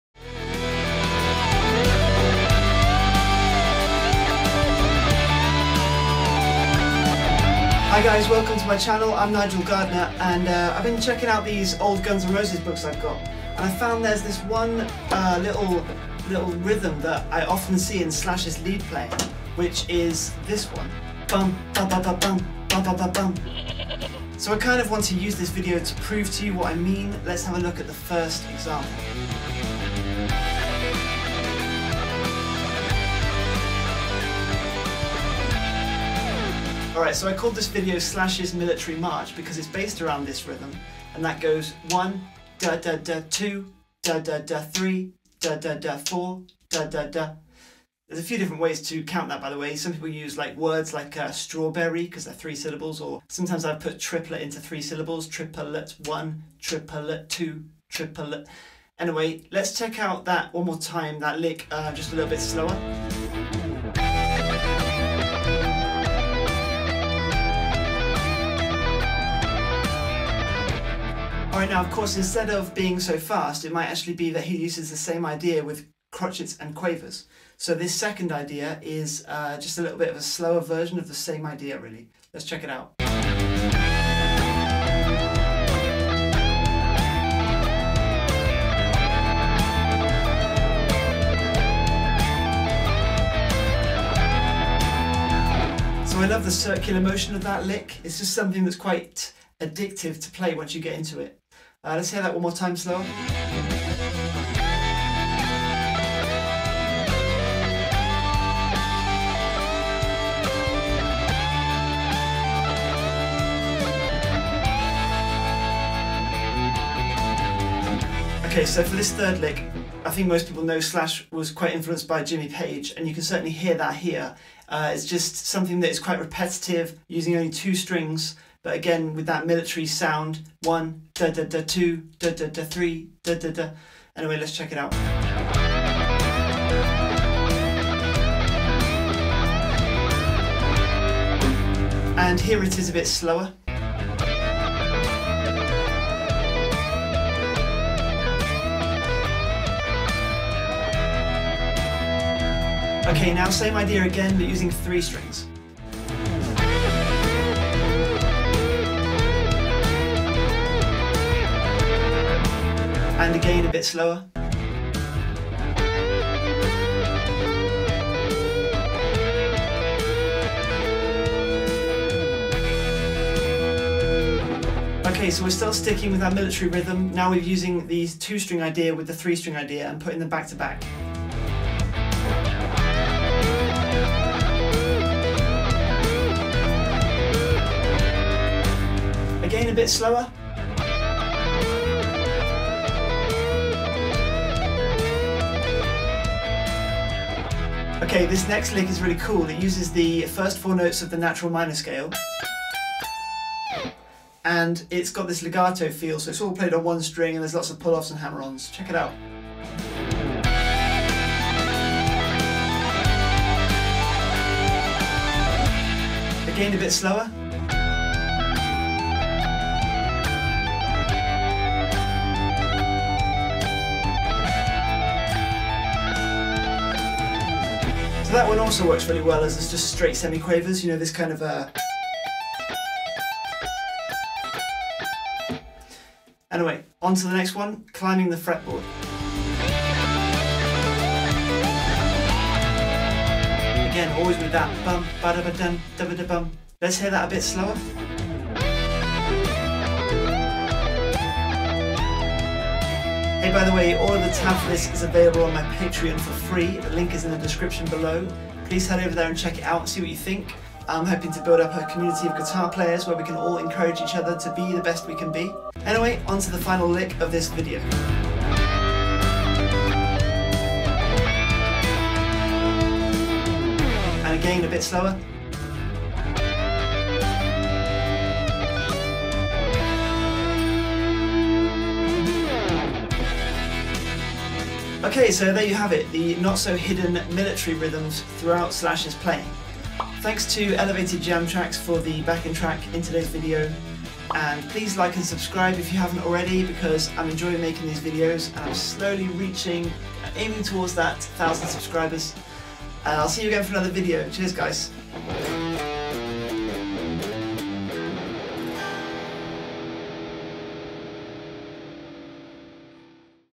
Learn these Licks - Slash’s *MILITARY* Secret
Here are a bunch of licks/phrases which Slash uses. They have one thing in common - they all use triplets. Specifically, a military style rhythm.

Hi, my name is Nigel Gardner! I'm a guitarist, teacher, and general music enthusiast from the UK! I'm keen to help anyone progress on their instrument - one video at a time!